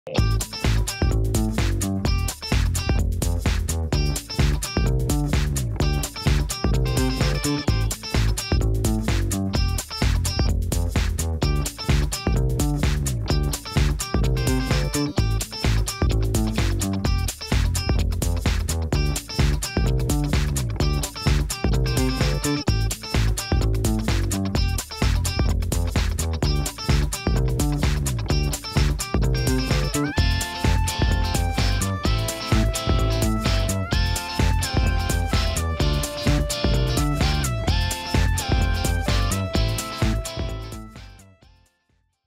Betting tips today | The best bets for Saturday 11th July
In this the best bets video series, we are presenting the top 3 betting tips today.
Get more hot betting tips for today by creating a free bettingexpert account on our website.

As always, we do ask you to gamble responsibly.